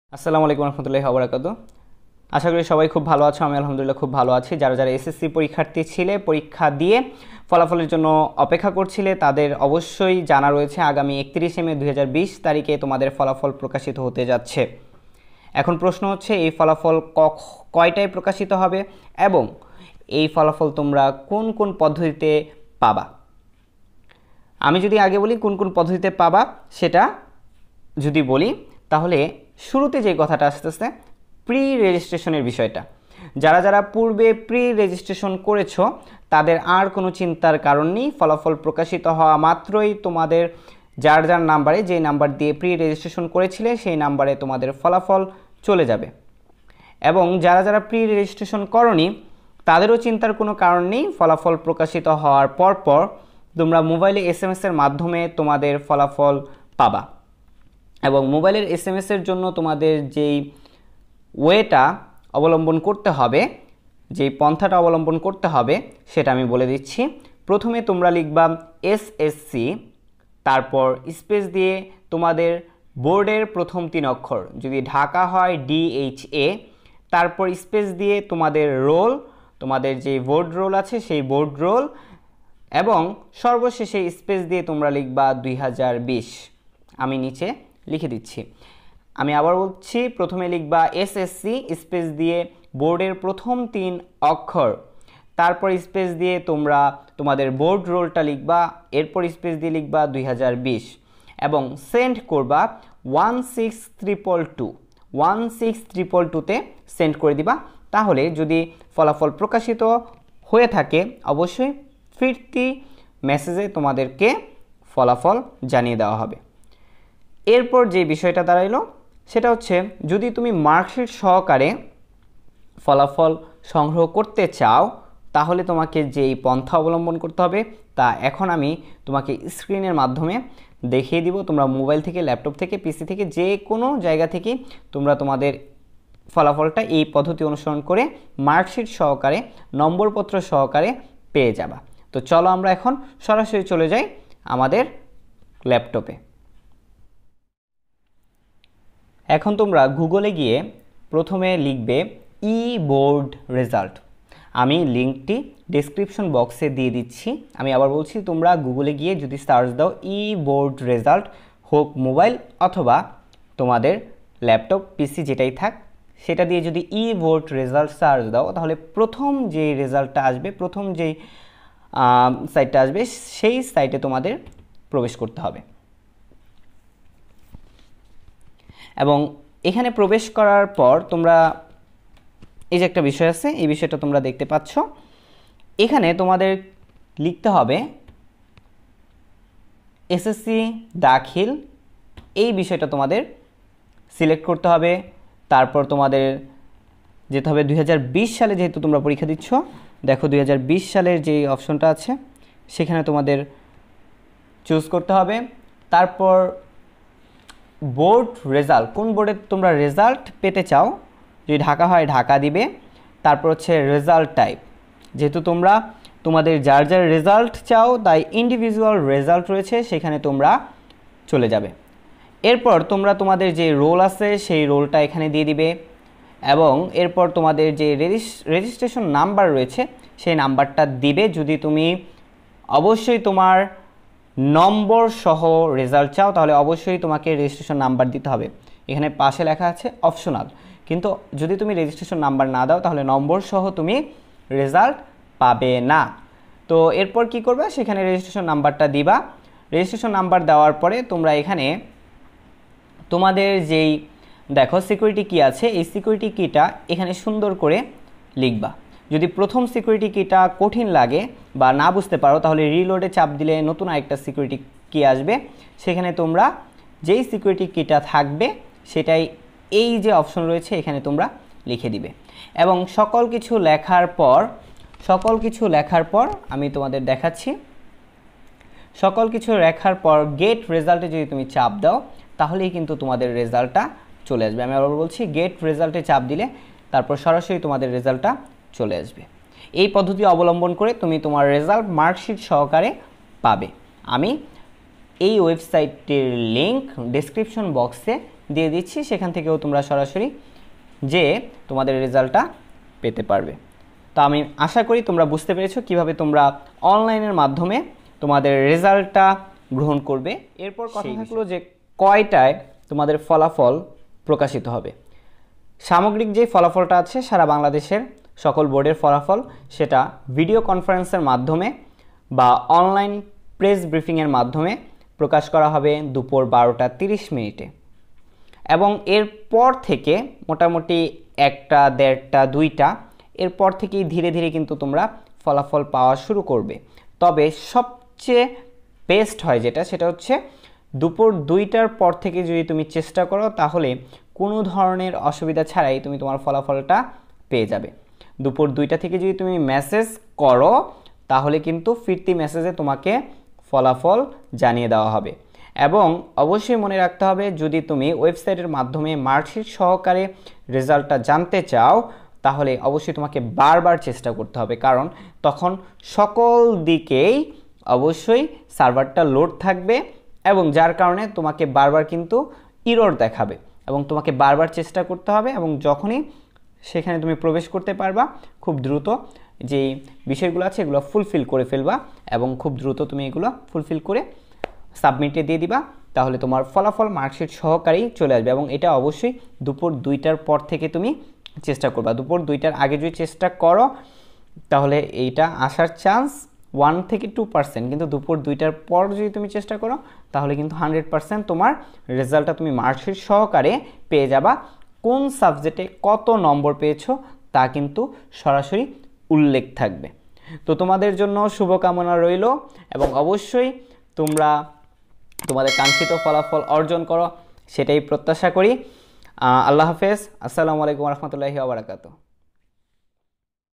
S.S.C Result 2020। এস.এস.সি রেজাল্ট সবার আগে মার্কশীটসহ।
আগামী ৩১শে মে ২০২০ তারিখে ফলাফল প্রকাশিত হবে।
যারা যারা প্রি-রেজিষ্ট্রেশন করেছো তারা সরাসরি রেজাল্ট পেয়ে যাবা। আর যারা করতে পারোনি তা রেজাল্ট প্রকাশিত হবার পর এস.এম.এস করে রেজাল্ট পাবা।